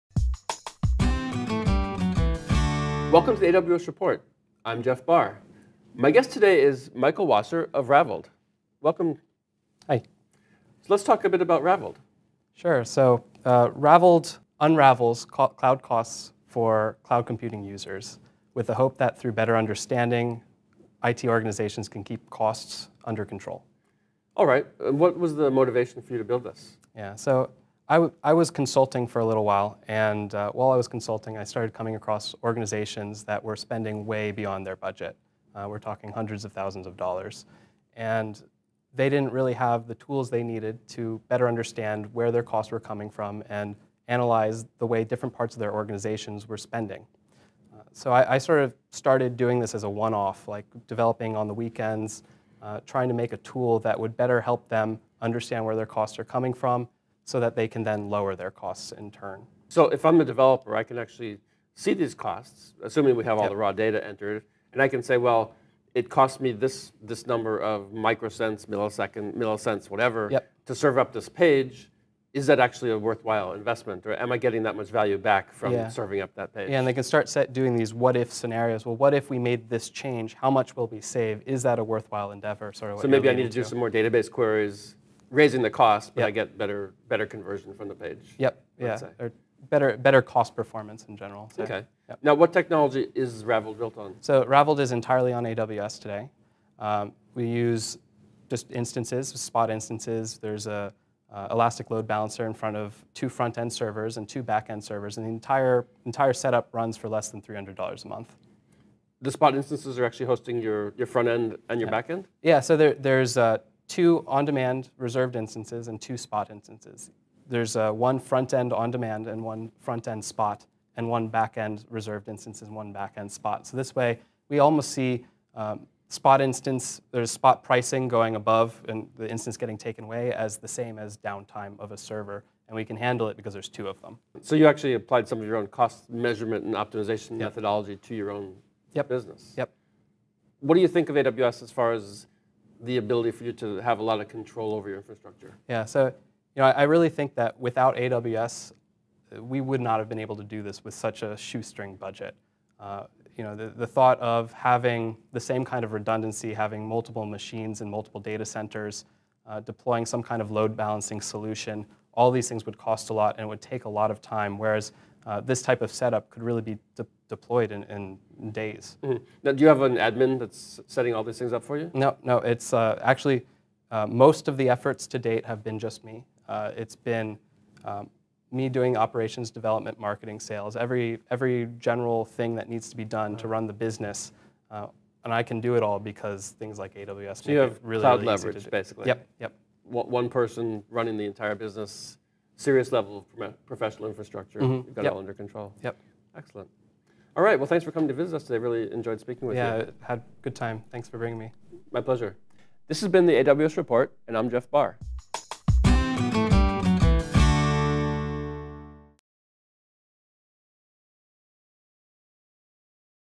AWS Report with Michael Wasser, Raveld
Michael Wasser, Founder of Raveld a startup in  Seattle,  talks about how he is using AWS products to run his business and help companies to cut operational costs.